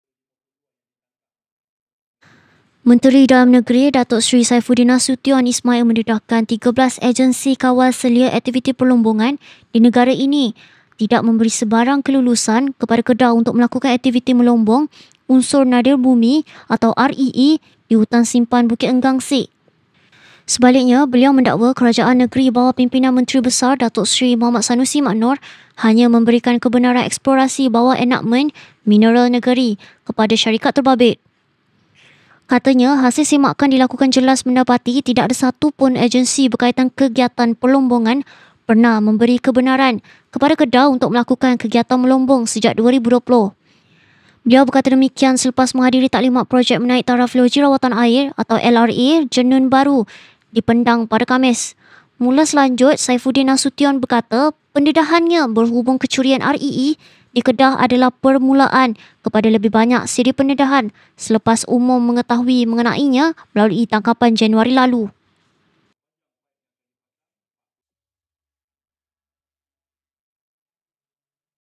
13 agensi kawal selia tidak beri kelulusan melombong REE di Kedah - Saifuddin Nasution
Menteri Dalam Negeri, Datuk Seri Saifuddin Nasution Ismail mendedahkan 13 agensi kawal selia aktiviti perlombongan di negara ini tidak memberi sebarang kelulusan kepada Kedah untuk melakukan aktiviti melombong unsur nadir bumi (REE) di Hutan Simpan Bukit Enggang, Sik.